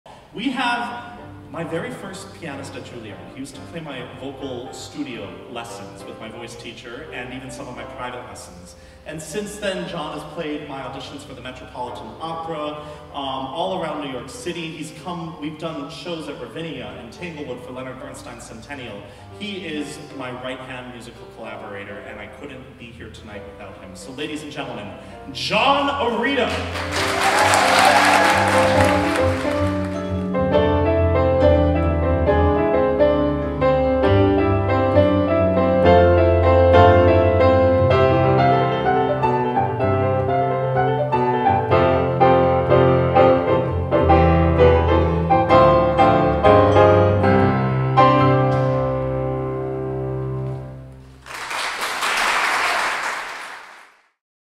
Highlighting John Arida
Pianist John Arida joined the Emberlight Festival's 2021 season for BROADWAY UNDER THE STARS and a classical recital with Emberlight's Artistic Director, Miles Mykkanen.

Pianist and coach John Arida has been praised as “an ardent accompanist… catching fire in the accompaniments and in the solo piece” by The Washington Post. His relationships with some of classical music’s most esteemed performers, including Julia Bullock, Anthony Roth Costanzo, Isabel Leonard, Megan Marino, Nicholas Phan, Ailyn Perez, and Sir James Galway, have led to international recital engagements across the United States, Canada, and Mexico. Notable venues include Carnegie Hall, The Metropolitan Museum of Art, and The Kennedy Center.

John begins his 2021 summer season with in-person concert engagements alongside Isabel Leonard, Julia Bullock, and Miles Mykkanen. Beyond the concert hall, John joins the music staff at Lakes Area Music Festival and is a member of the inaugural artistic team at Emberlight Festival, spearheaded by tenor Miles Mykkanen.

Mr. Arida has assisted notable conductors John DeMain, Evan Rogister, Speranza Scappucci, Emmanuel Villaume amongst others. He has been a member of the music staff of several U.S. companies and festivals including the Juilliard School, Washington National Opera, Central City Opera.

John Arida has held fellowships at the Aspen Music Festival and the Music Academy of the West. He holds a master’s degree in collaborative piano from The Juilliard School.